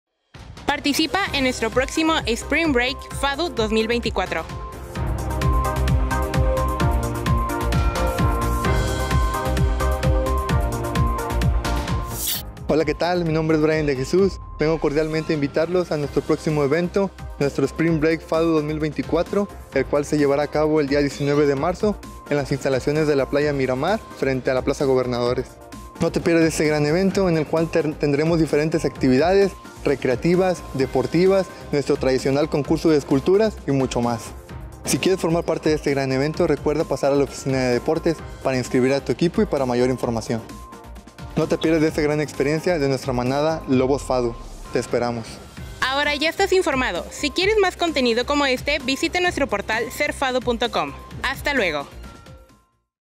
Participa en el Spring Break FADU 2024
Con el objetivo de promover la sana convivencia y el deporte en la comunidad estudiantil la Facultad de Arquitectura, Diseño y Urbanismo de la Universidad Autónoma de Tamaulipas, participa en nuestro próximo evento deportivo “Spring Break FADU 2024”.

Tan GRANDE como quieras ser, ser FADU, sé UAT.

Visitanos y conoce por medio de postales y reels a nuestra comunidad FADU: